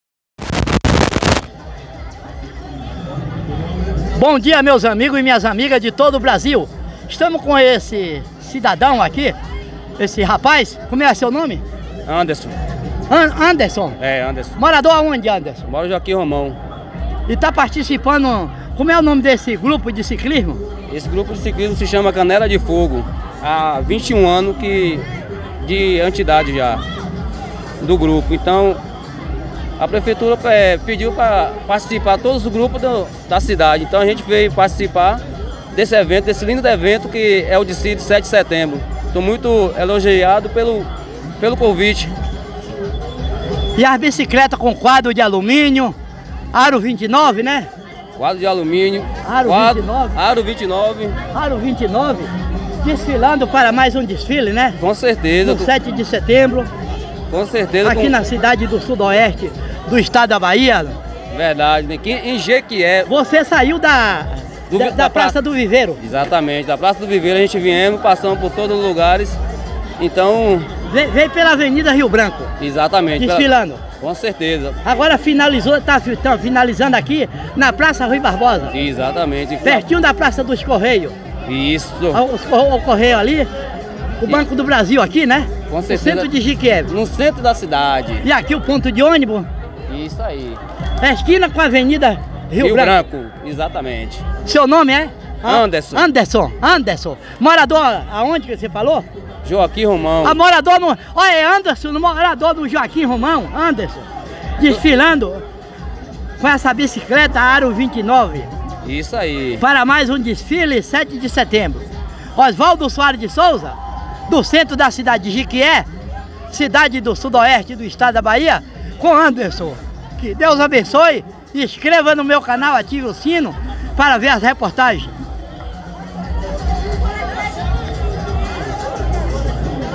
VN NOTÍCIAS REPÓRTER NO DESFILE 7 DE SETEMBRO DE 2024 EM JEQUIÉ BAHIA BRASIL
ESTAMOS APRESENTANDO  ESTE  DESFILE  DO 7 DE SETEMBRO DE 2024 FESTA  DA INDEPENDÊNCIA  DO BRASIL  NA AVENIDA RIO BRANCO  CENTRO DE JEQUIÉ CIDADE DO SUDOESTE DO ESTADO DA BAHIA BRASIL

VARDINHO REPÓRTER
YOUTUBE VN NOTÍCIAS
CARGO DE CINEGRAFISTA
CACHEIRO VIAJANTE